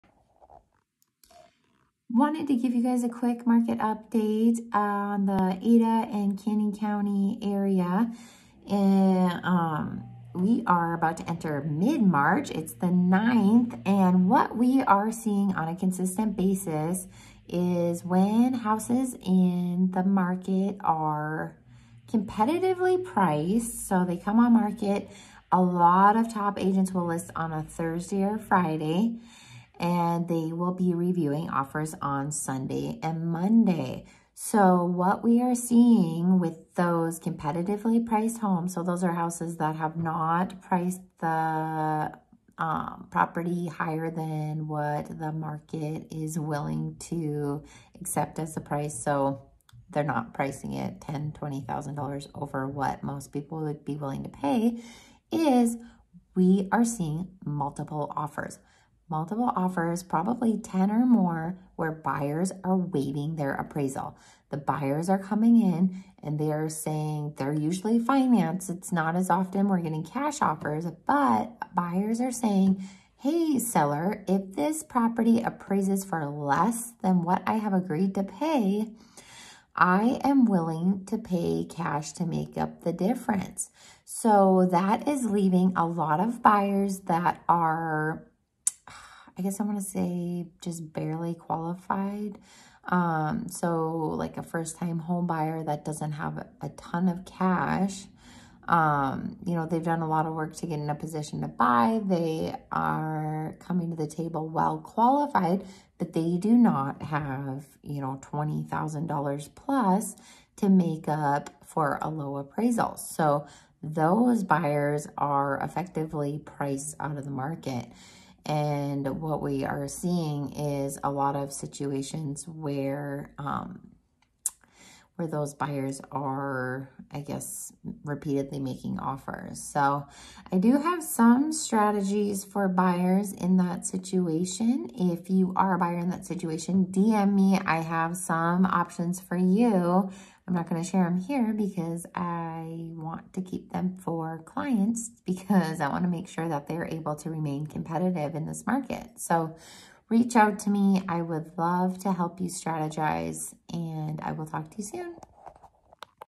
Mid March 2022 BOOTS ON THE GROUND Boise Real Estate Market Update
Ada & Canyon County mid March update 🚧

Here’s a BOOTS ON THE GROUND perspective of what’s happening for BUYERS in today’s hot spring market.

I have some winning strategies to get you in the perfect home for you before summer ☀️😎